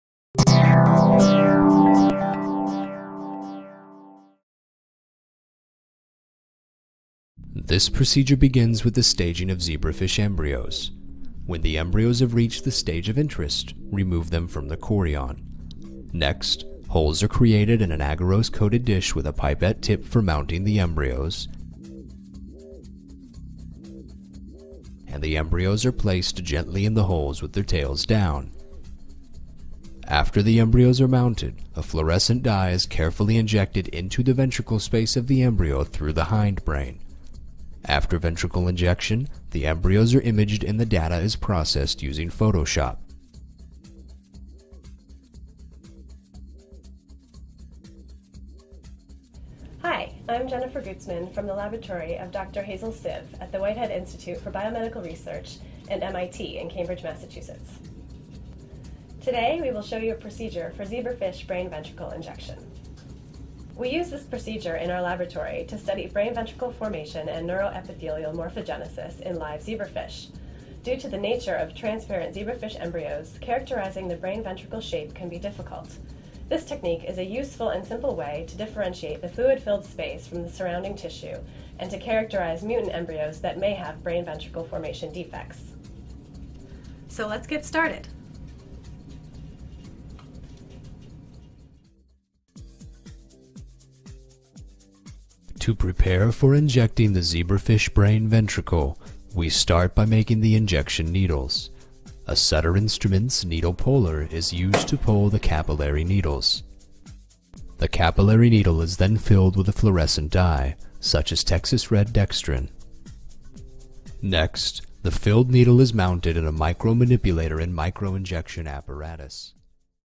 Zebrafish Brain Ventricle Injection l Protocol Preview
Zebrafish Brain Ventricle Injection -  a 2 minute Preview of the Experimental Protocol

Jennifer H. Gutzman, Hazel Sive
Whitehead Institute for Biochemical Research,; MIT - Massachusetts Institute of Technology,;

After neural tube formation, the neuroepithelium constricts and folds while the tube fills with embryonic cerebrospinal fluid (eCSF) to form the embryonic brain ventricles.  We developed this ventricle injection technique to better visualize the fluid filled space in contrast to the neuroepithelial shape in a live embryo.